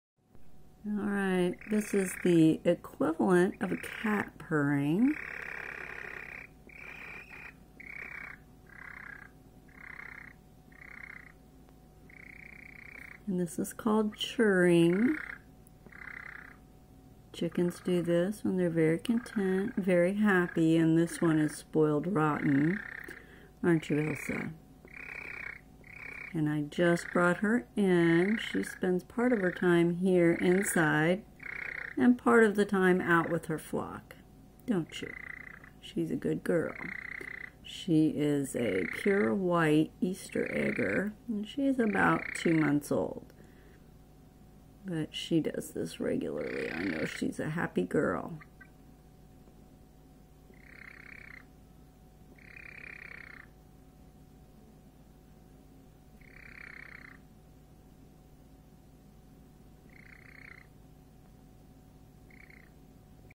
Cute Churring Chicken
Elsa gives us a nice churring trill, indicating her happiness.